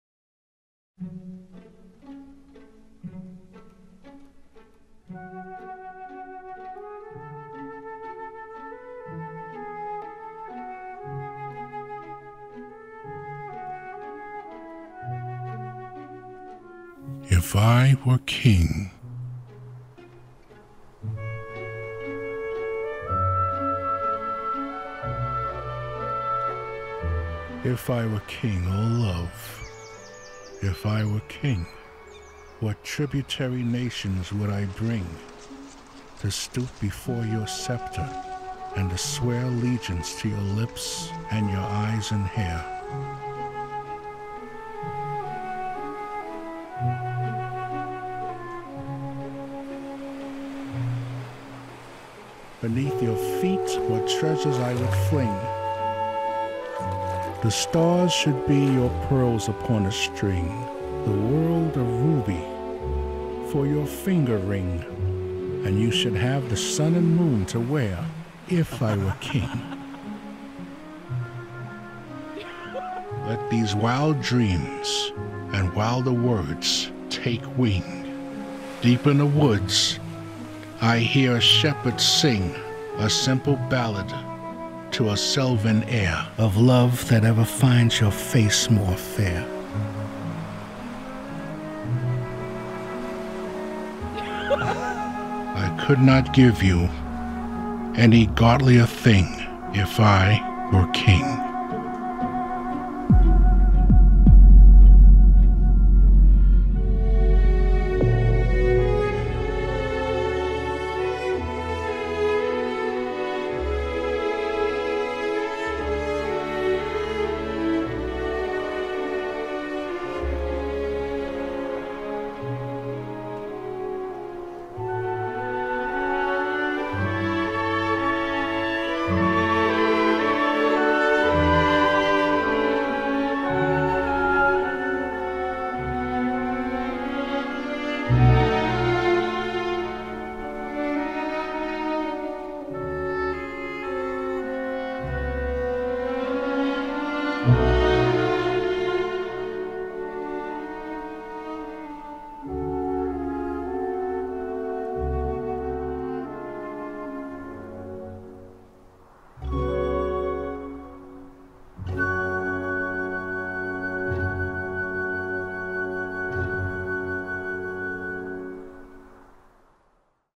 MY RODE REEL 2018:JUSTIN HUNTLY McCARTHY'S IF I WERE KING (2018)
If I Were King
It's a tone-poem piece read by myself with
a classic music soundtrack and stock footage
images w/ visual graphical effects l entered
in the MY RODE MICROPHONE REEL 2018 short video
film contest this past summer.
I believe you will enjoy it and if possible
share it with others you believe would be
interested.

If I were king­­­ ah love, if I were king!
What tributary nations would I bring
To stoop before your scepter and to swear
Allegiance to you lips and eyes and hair.
Beneath your feet what treasures I would fling:­­­
The stars should be your pearls upon a string,
The world a ruby for your finger ring,
And you should have the sun and moon to wear
Let these wild dreams and wilder words take wing,
Deep in the woods I hear a shepherd sing
A simple ballad to a sylvan air,
Of love that ever finds your face more fair.
I could not give you any godlier thing
- Justin Huntly MCCarthy